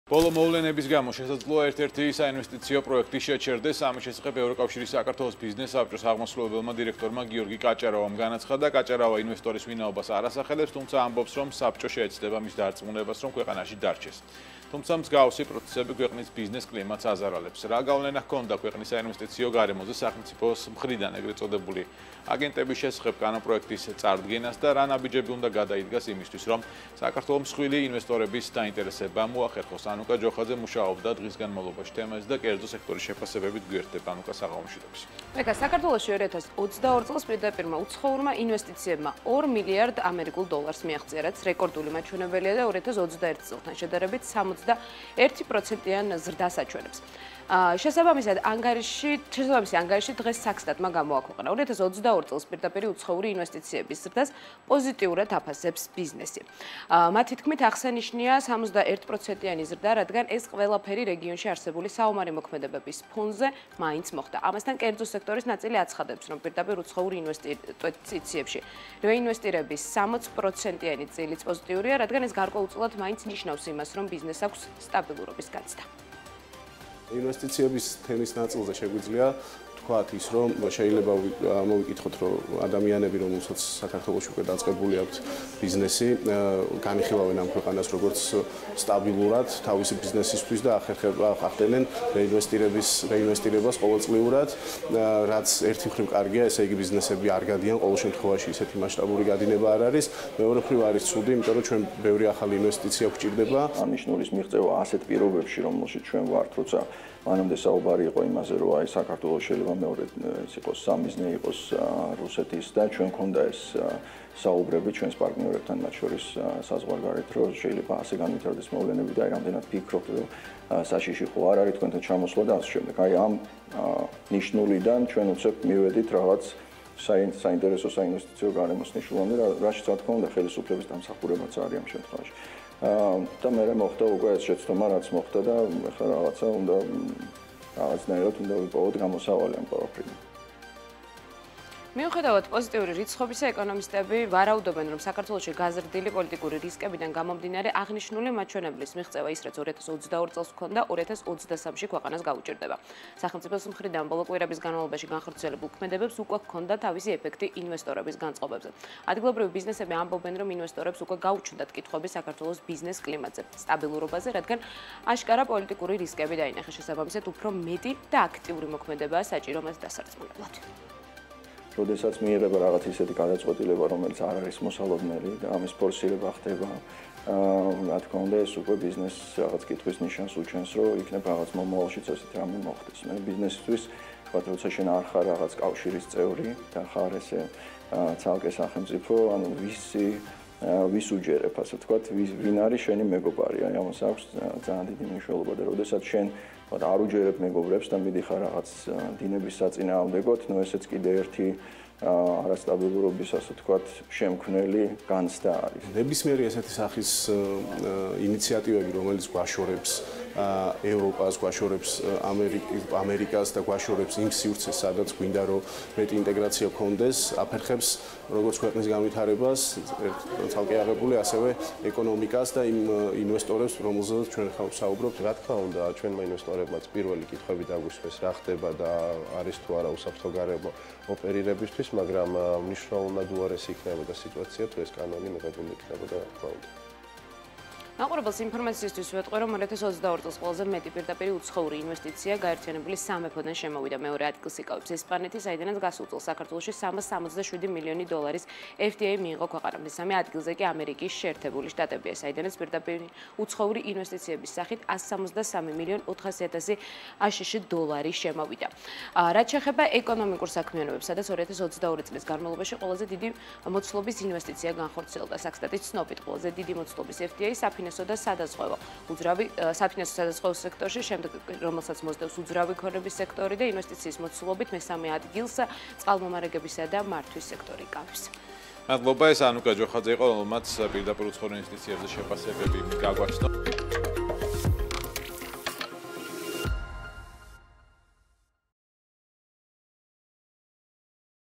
2022 წელს საქართველოში FDI-მ $2 მილიარდს მიაღწია - ბიზნესის შეფასება
ბოლო მოვლენების გამო, შესაძლოა, ერთ-ერთი საინვესტიციო პროექტი შეჩერდეს” - ამის შესახებ ევროკავშირ-საქართველოს ბიზნეს-საბჭოს აღმასრულებელმა  დირექტორმა  გიორგი კაჭარავამ განაცხადა. კაჭარავა ინვესტორის ვინაობას არ ასახელებს, თუმცა ამბობს, რომ  საბჭო შეეცდება მის დარწმუნებას, რომ ქვეყანაში დარჩეს.  თუმცა მსგავსი პროცესები ქვეყნის ბიზნეს კლიმატს აზარალებს. რა გავლენა ჰქონდა ქვეყნის საინვესტიციო გარემოზე სახელმწიფოს მხრიდან ე.წ აგენტების შესახებ კანონპროექტის წარდგენას და რა ნაბიჯები უნდა გადადგას იმისთვის, რომ საქართველომ მსხვილი ინვესტირების დაინტერესება შეძლოს?